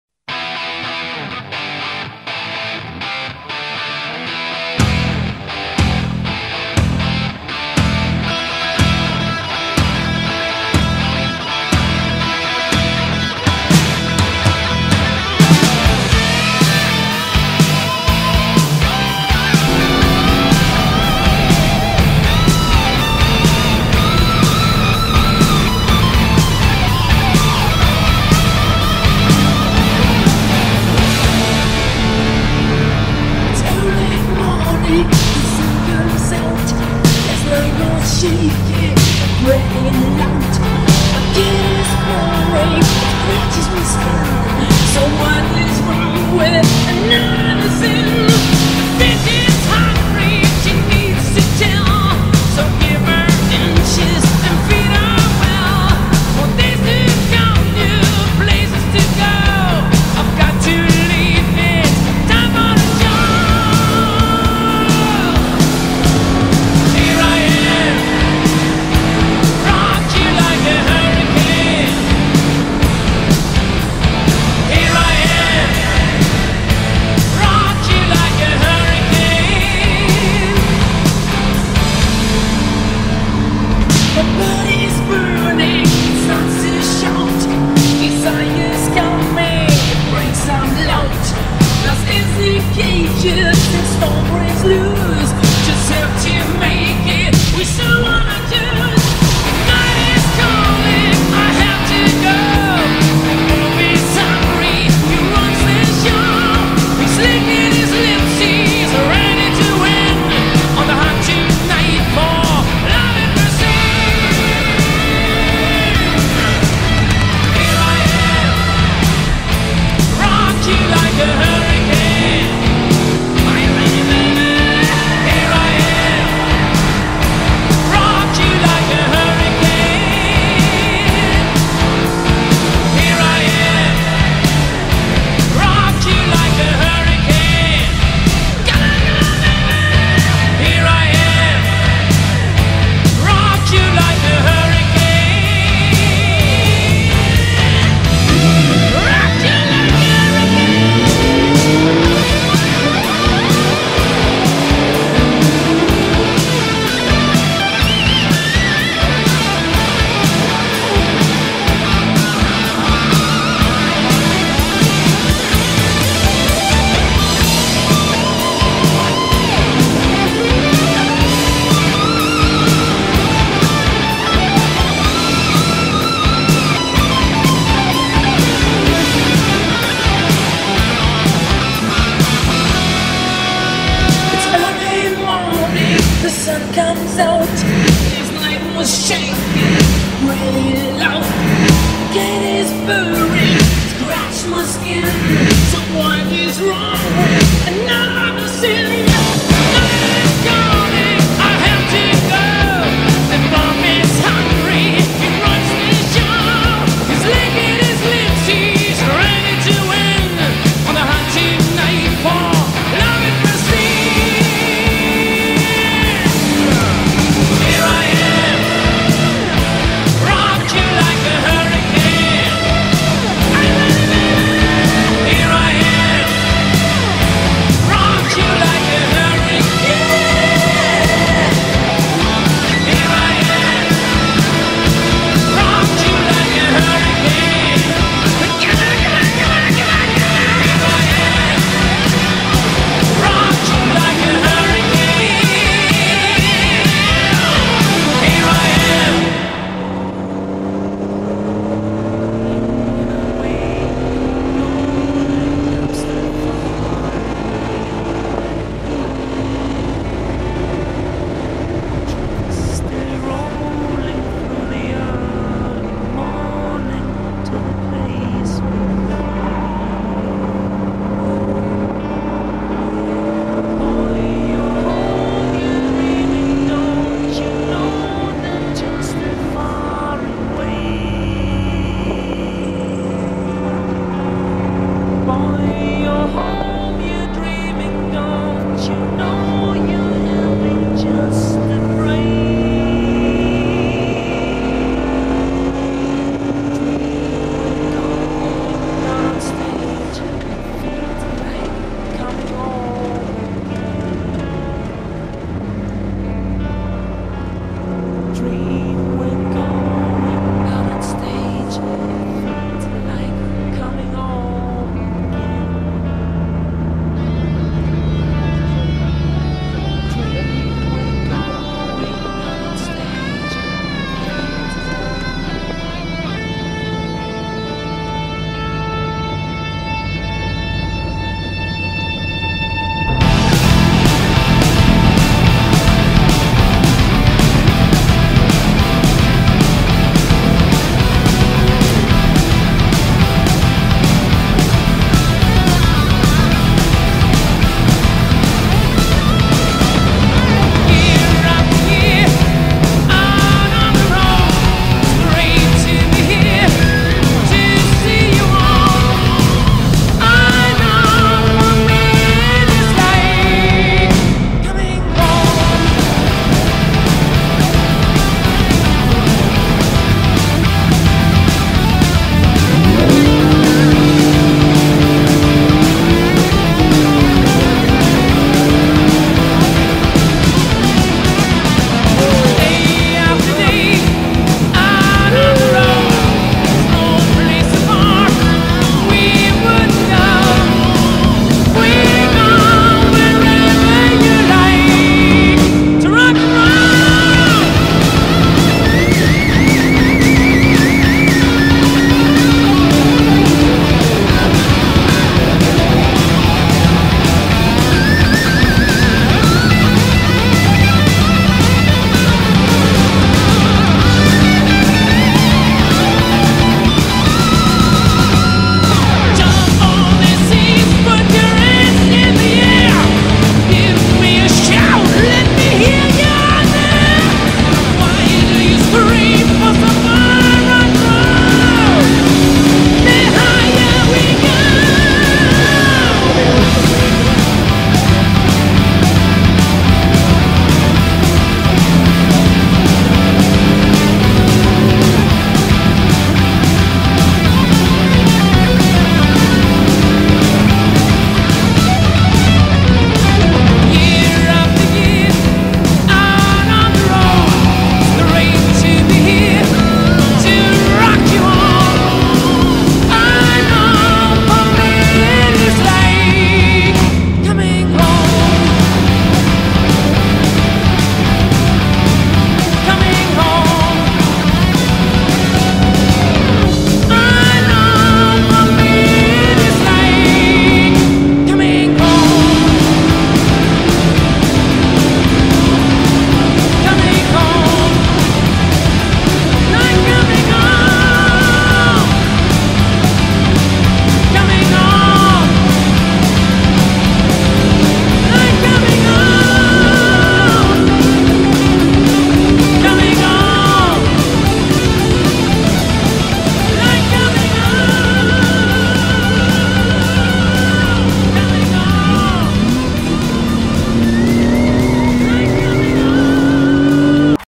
Alfa Romeo GTV6 in Brno
We spent a nice trackday for Alfas on the Brno racetrack